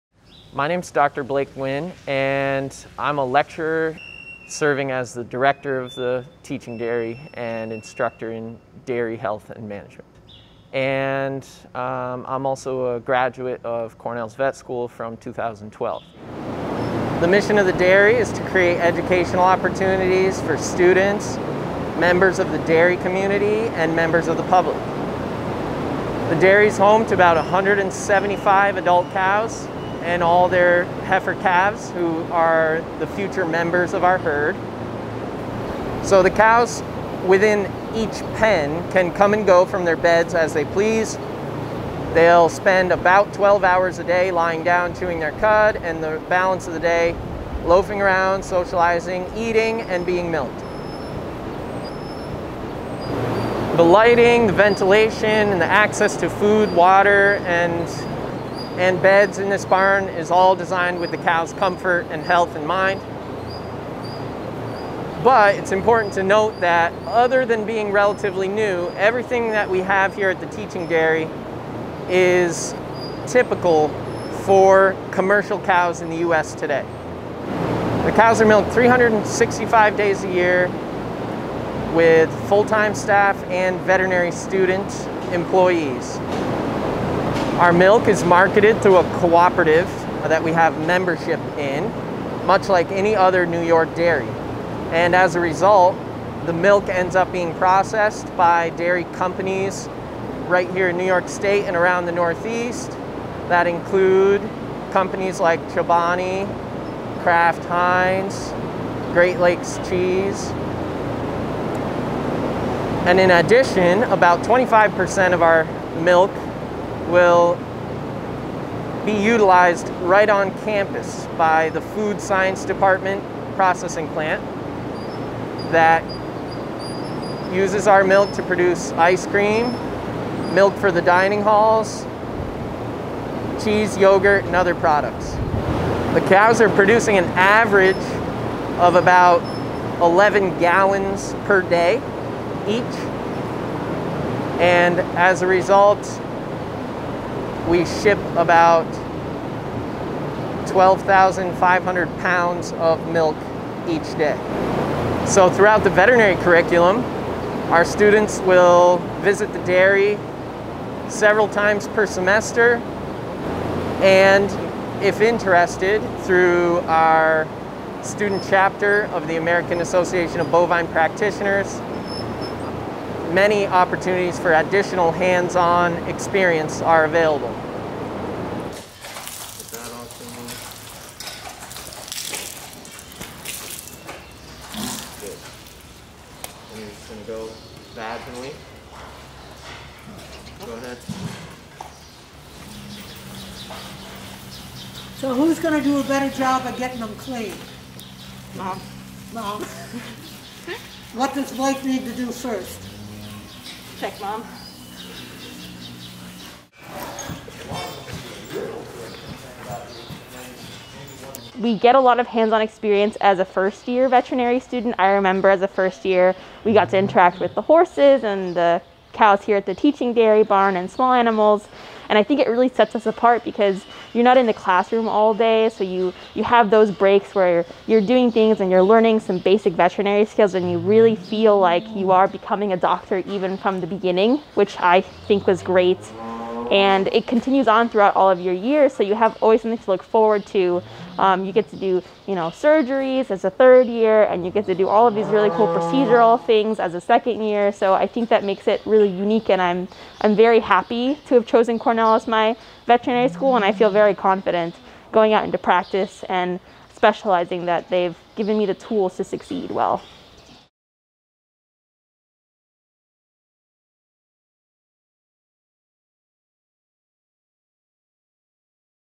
Overview of the Cornell Teaching Dairy Barn and student training
Lecturer and director of the Cornell Teaching Dairy Barn Blake Nguyen, D.V.M. '12, gives an overview of their operations, with Jennifer Rodriguez-Diaz, Class of 2021, describing the student experience.

Video: Carol Jennings/CornellVet